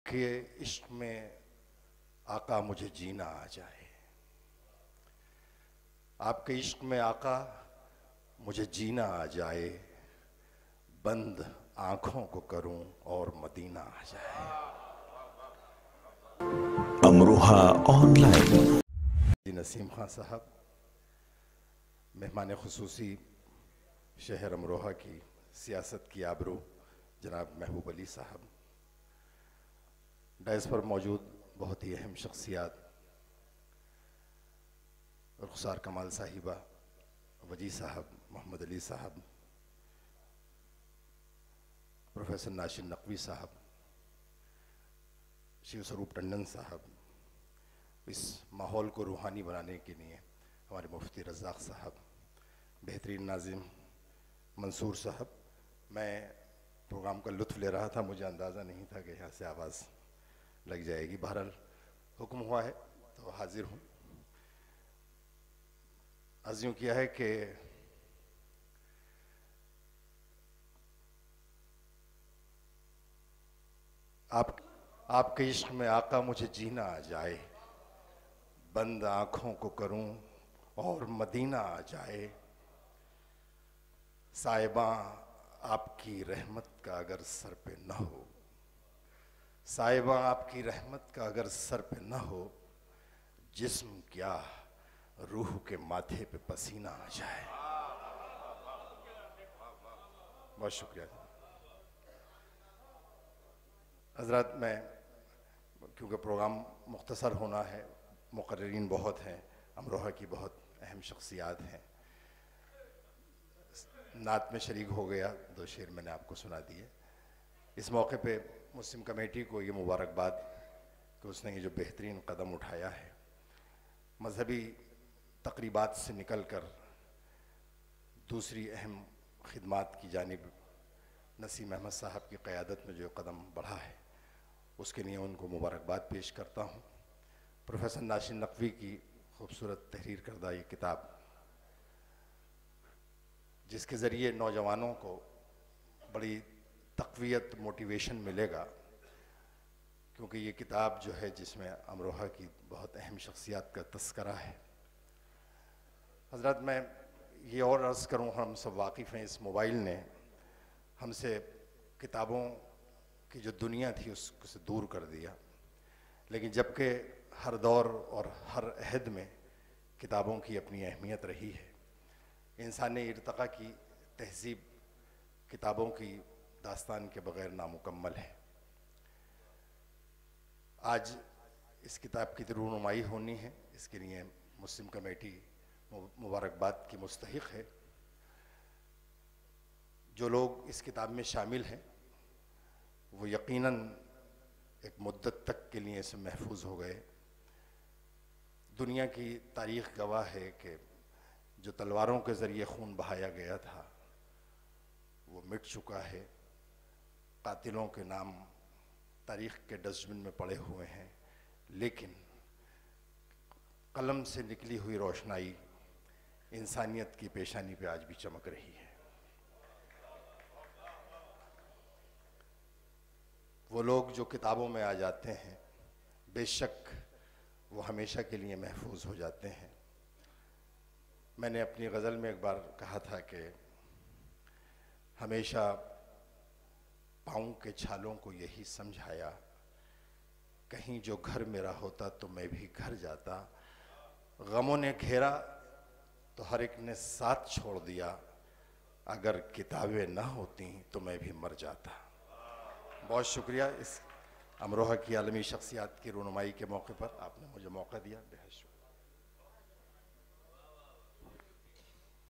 अमरोहा न्यूज़ | Dr. Jamshed congratulated the Muslim Committee Kamal Principal at I M Inter College,
Jamshaid Kamal Amrohvi  ने दी मुस्लिम कमेटी को बधाई

विश्व विख्यात वैज्ञानिक श्री वजीह अहमद नकवी ने कहा कि मैं आज जो कुछ भी हु अमरोहा की देन हु और मुझे अमरोहवी होने पर फख्र है, साथ ही साथ उन्होंने मुस्लिम कमेटी अमरोहा का शुक्रिया अदा किया।
मोहतरमा रुखसार कमाल ने कहा कि मेरे पिता कमाल_अमरोहवी को अमरोहा की मिट्टी से बहुत मुहब्बत थी और मुझे नही मालूम था कि अमरोहा की अवाम मेरे वालिद जनाब कमाल अमरोहवी साहब को इतना प्यार करती है, जिसका मुझे आज इस प्रोग्राम में आने पर एहसास हुआ।

इसी बीच शिक्षा के मैदान में मुस्लिम कमेटी द्वारा हाशमी गर्ल्स डिग्री कालिज, नायाब अब्बासी डिग्री कालिज, राशिदा बेगम डिग्री कालिज, जे0 एस0 डिग्री कालिज, उमापति महा विद्यालय, साहस डिग्री कालिज आदि शिक्षण संस्थानों के प्रबंधकों को एवार्ड से नवाजा गया।
कार्यक्रम में मुख्य रूप से प्रो0 नाशिर नकवी, डॉ महताब अमरोहवी, हाजी खुर्शीद अनवर, सरताज मंसूरी, ओवेस रिज़वी आदि ने अपने विचार व्यक्त किये।
कार्यक्रम का संचालन मंसूर अहमद एड0 ने किया व आखीर में अध्यक्ष हाजी नसीम खा ने मुस्लिम कमेटी की ओर से सभी का धन्यवाद दिया।

World renowned scientist  Wajih Ahmed Naqvi